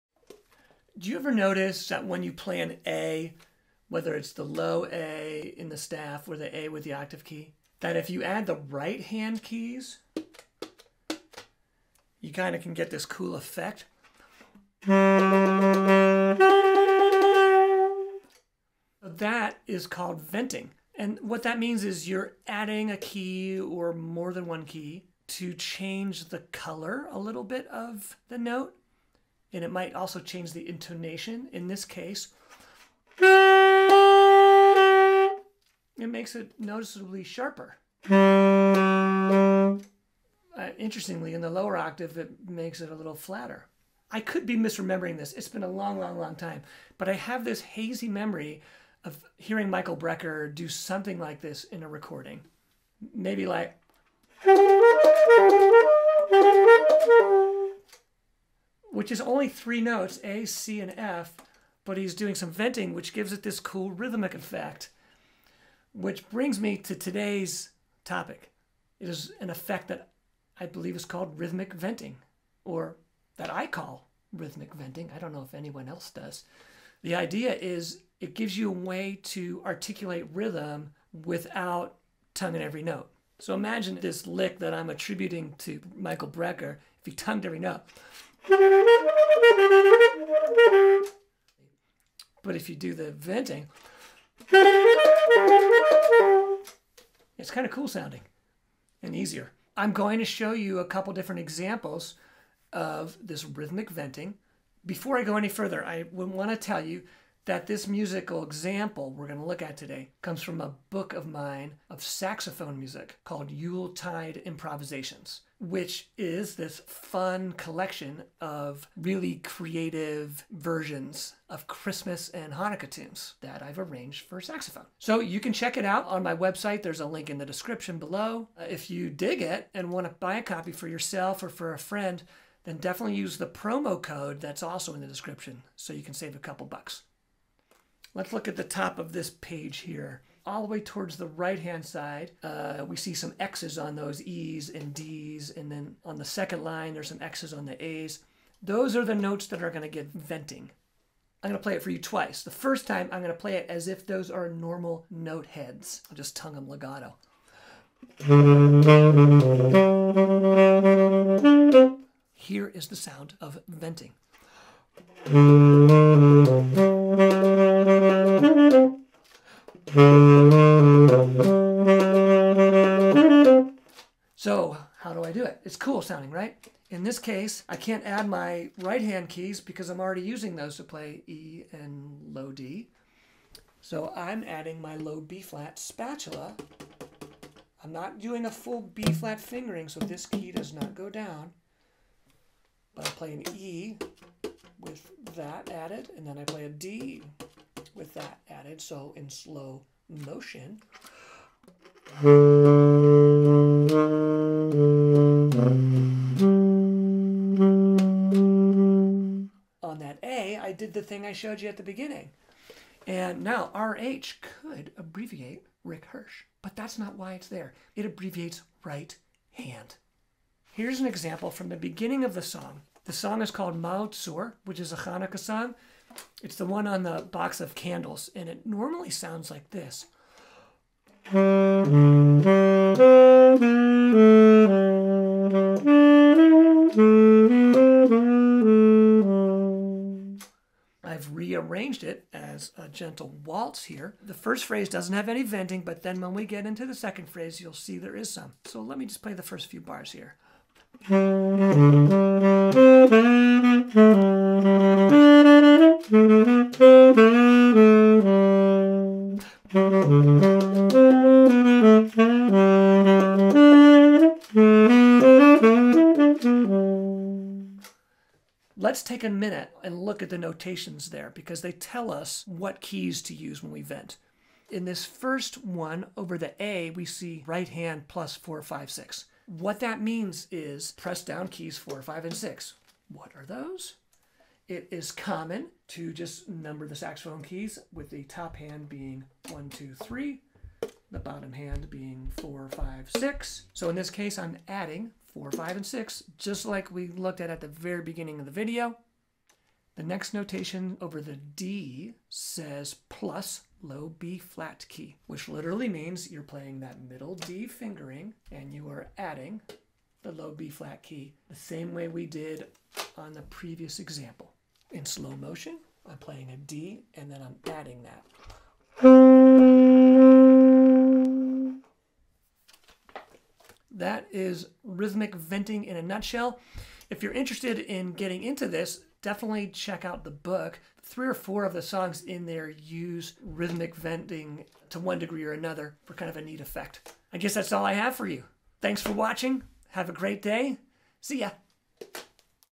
Are you hip to this saxophone technique?
Learn Rhythmic Venting, a very hip extended technique on saxophone. It's a novel way to articulate rhythm, and adds interesting color to your lines.

0:00 Intro and demonstration
0:44 Brecker-ish example
2:18 Venting example #2
3:41 Venting example #3 and how to read the alternate notation